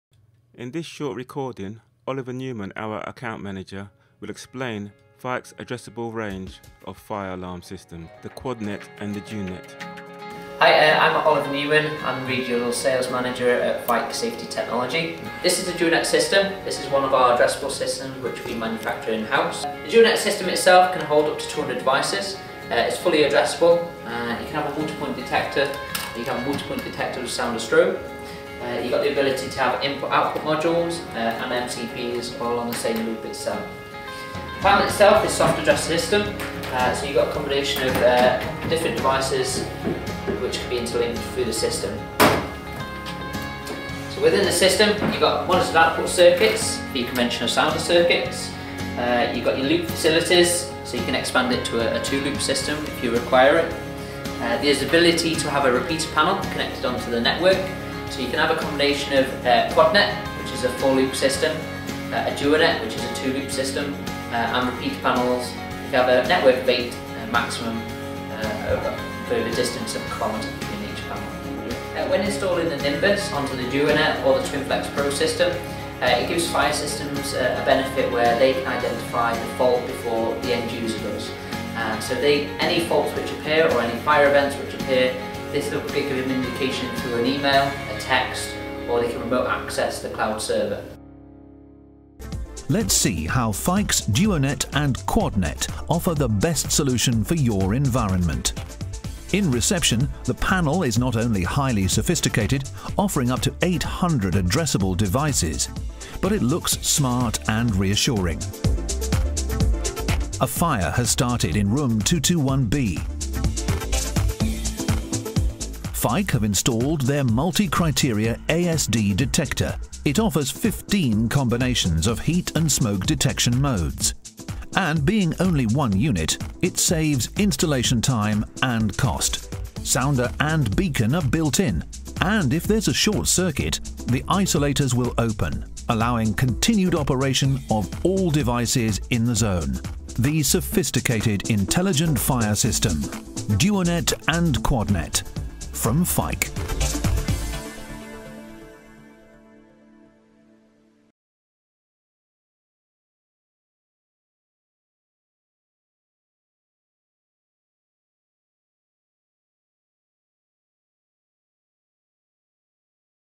Quadnet & Duonet Fire Alarm
This video provides a summary of the addressable fire alarm systems the Quadnet & Duonet from Fike. Oliver Newman from Fike explains, followed by seeing the fire alarm system in action.